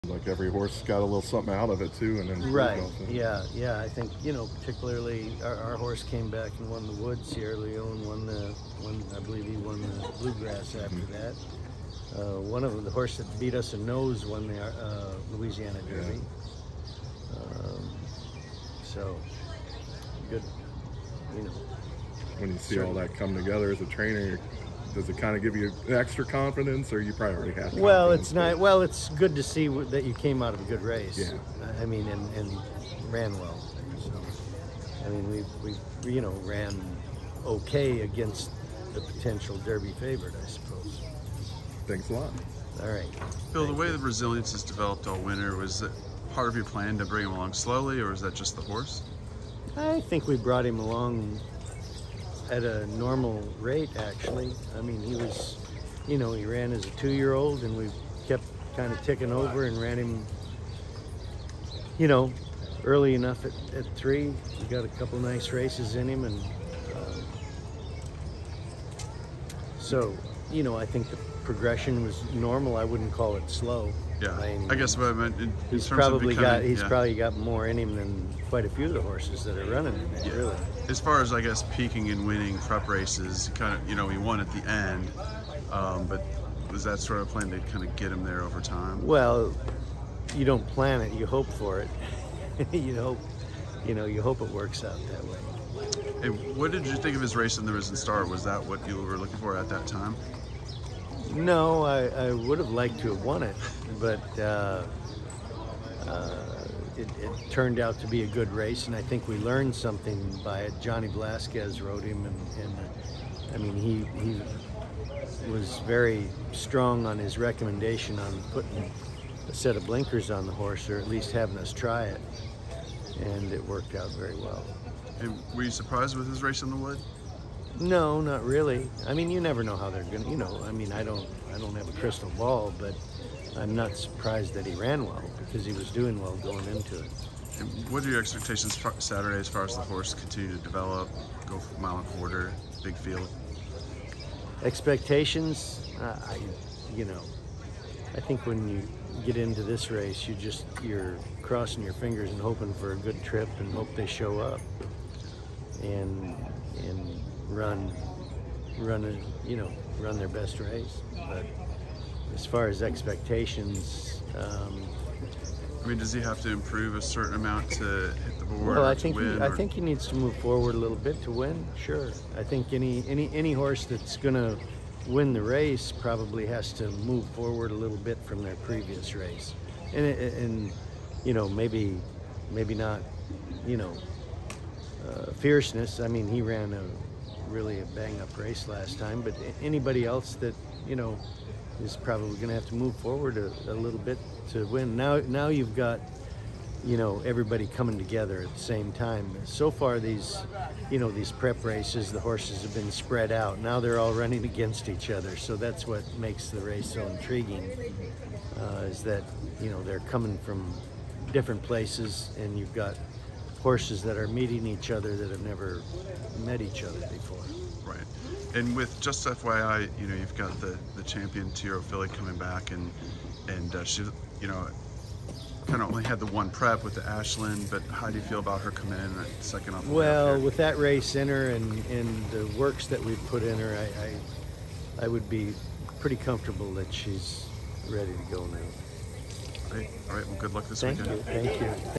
Kentucky Derby 2024 - Trainer Bill Mott on Resilience (#19)
Trainer Bill Mott on Resilience as he gets ready for Kentucky Derby 150!